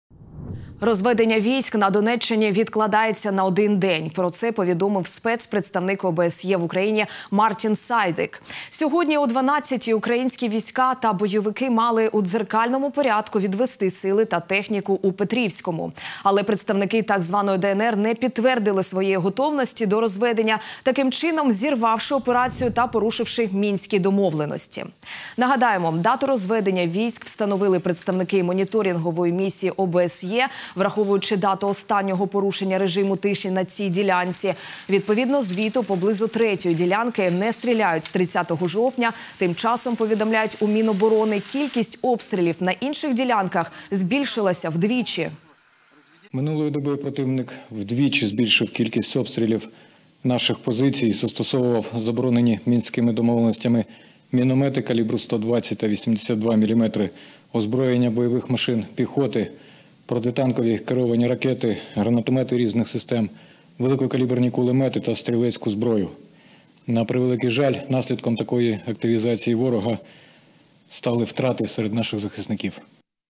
Розведення військ на Донеччині відкладається на один день
Про це повідомив спецпредставник ОБСЄ в Україні - Мартін САйдік. Сьогодні о 12.00 Українські війська та бойовики мали у дзеркальному порядку відвести сили та техніку у Петрівському. Але представники так званої ДНР не підтвердили своєї готовності до розведення, таким чином, зірвавши операцію та порушивши мінські домовленості. Нагадаємо, дату розведення військ встановили представники моніторингової місії ОБСЄ. Враховуючи дату останнього порушення режиму тиші на цій ділянці. Відповідно звіту , поблизу третьої ділянки не стріляють з 30 жовтня. Тим часом, повідомляють у Міноборони, кількість обстрілів на інших ділянках збільшилася вдвічі.